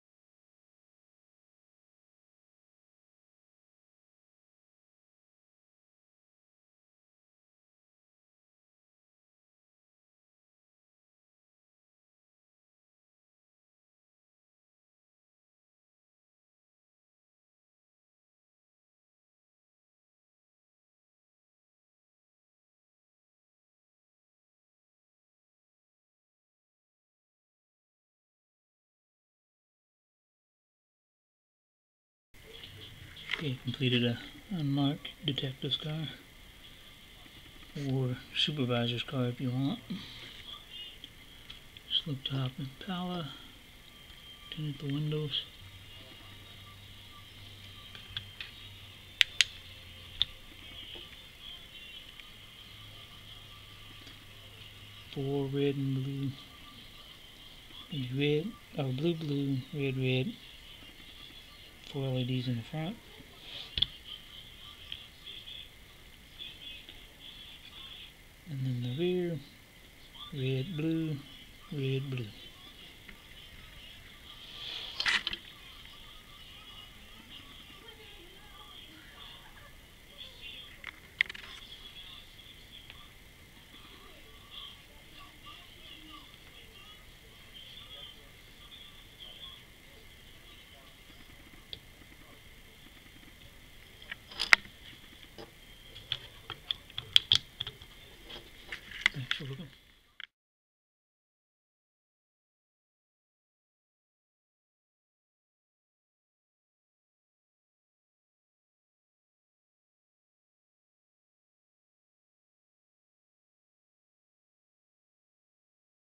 1/32 POLICE unmarked impala slicktop with leds
Completed a slicktop impala with leds. 4 leds on rear deck(2-red 2 - blue) and 4 leds on front visor center (2- red 2-blue). Windows were also tinted. Thanks for looking.